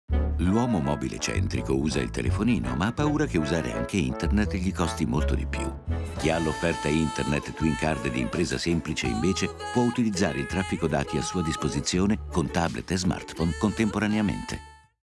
INTERNET TWIN CARD di Impresa Semplice -- spot 15"
Con Internet Twin Card all'interno dell'offerta Tutto è possibile condividere i gigabite a disposizione contemporaneamente con tablet e smartphone, alla velocità del 4G. Info qui